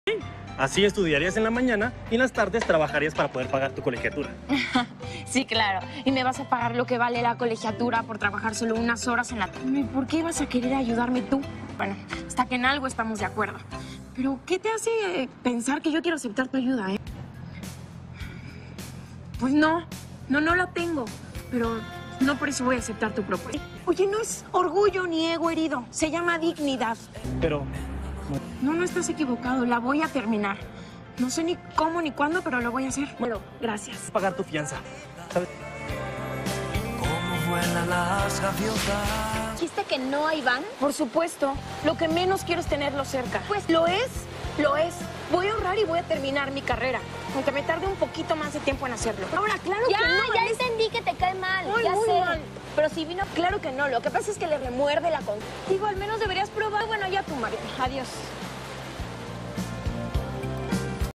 354
Cortesía darkhaven. Toda una belleza Renata Notni, si bien no enseña como otras, la verdad es que me conformo con poder admirarla. Sí está lindísima.